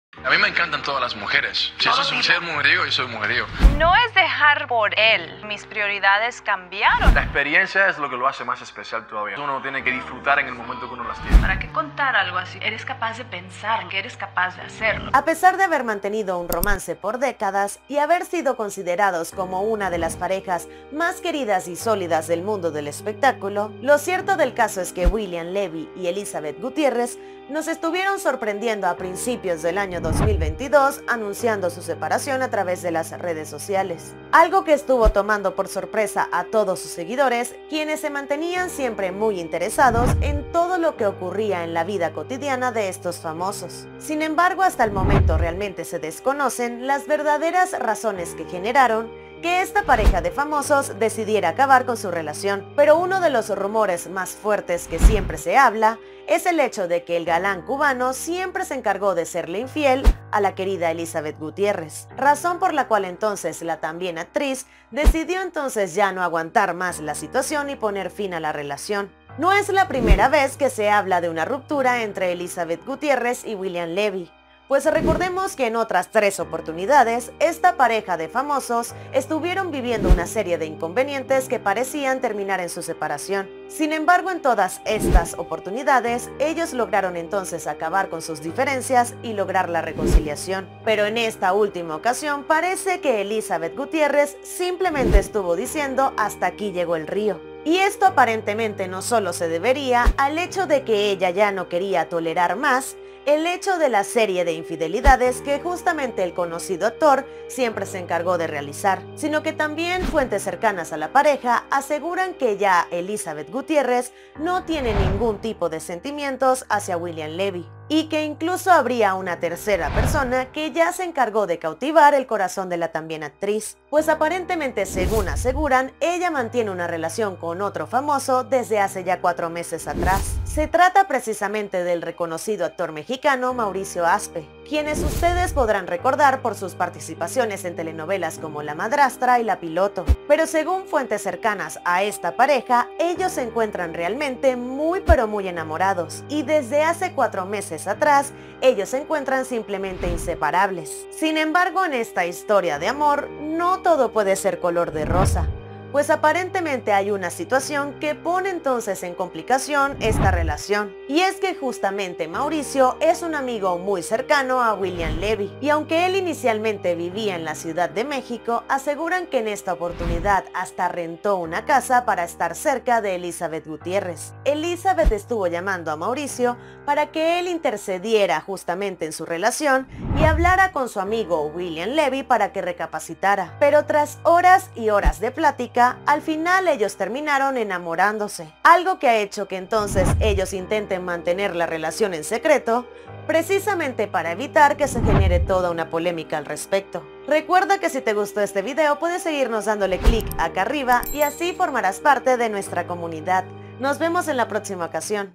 ⛔ Revelan el 𝗻𝘂𝗲𝘃𝗼 𝗿𝗼𝗺𝗮𝗻𝗰𝗲 de Elizabeth Gutiérrez / Ya quedó en el olvido William Levy
La actriz ya tendría una relación de meses con famoso actor…